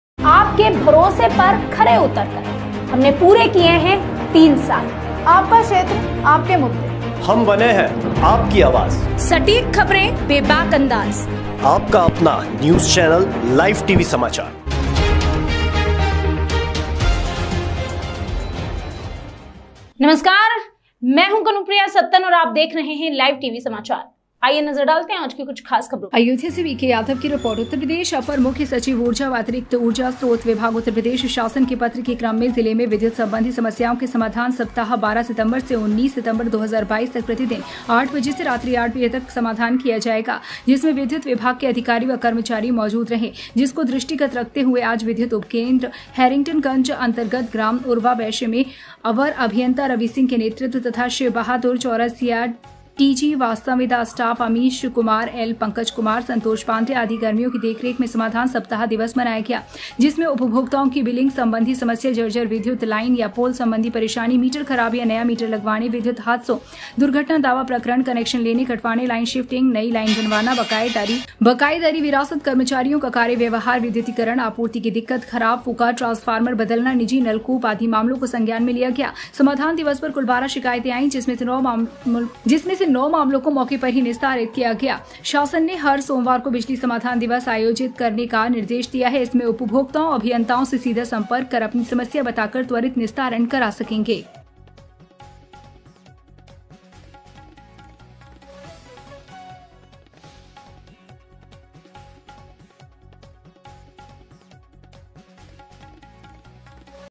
अयोध्या : थाना और तहसील समाधान दिवस के बाद शासन ने बिजली समाधान दिवस का आयोजन
लोकेशन हैरिग्टनगंज
रिपोर्ट वी के यादव ब्यूरो अयोध्या
*थाना और तहसील समाधान दिवस के बाद शासन ने बिजली समाधान दिवस का आयोजन कराने का निर्णय लिया है। अब इसके जरिए समस्याओं के निस्तारण के लिए बनाई गई रुपरेखा*

उत्तर प्रदेश अपर मुख्य सचिव,ऊर्जा एवं अतिरिक्त ऊर्जा स्रोत विभाग,उ0प्र0 शासन के पत्र के क्रम जिले मे विद्युत सम्बन्धी समस्याओं हेतु “समाधान सप्ताह”12 सितम्बर से 19 सितम्बर 2022 तक प्रतिदिन प्रातः 08:00 बजे से रात्रि 08:00 बजे तक समाधान किया जायेगा
जिसमे विधुत विभाग के अधिकारी व क्रमचारी मौजूद रहे जिसको दृष्टि गत रखते हुऐ आज विधुत उपकेंद्र हैरिग्टनगंज अन्तर्गत ग्राम उरुवा वैश्य मे अवर अभियंता रवी सिंह के नेत्रत्व तथा शिव बहादुर चौरसिया टी जी व संविदा स्टाफ अमीष कुमार एल,पंकज कुमार,संतोष पान्डेय, आदि क्रमियो की देखरेख मे समाधान सप्ताह दिवश मनाया गया जिसमे
उपभोक्ताओं की बिलिग संबंधी समस्या,जर्जर विद्युत लाइन या पोल संबंधी परेशानी,मीटर खराब या नया मीटर लगवाने,विद्युत हादसों, दुर्घटना दावा प्रकरण, कनेक्शन लेने,कटवाने, लाइन शिफ्टिग,नई लाइन बनवाना, बकाएदारी,वरासत, कर्मचारियों का कार्य व्यवहार,विद्युतीकरण, आपूर्ति की दिक्कत खराब,फुंका ट्रांसफार्मर बदलना,निजी नलकूप,
आदि मामलों को संज्ञान मे लिया गया आज समाधान दिवश पर कुल बारह सिकायते सामने आयी जिसमे नौ मामलो को मौके पर निस्तारण किया गया शासन ने हर सोमवार को बिजली समाधान दिवस का आयोजन कराने का निर्देश दिया है। इसमें उपभोक्ता अभियंताओं से सीधे संपर्क कर अपनी समस्या बताकर त्वरित निस्तारण करा सकेंगे।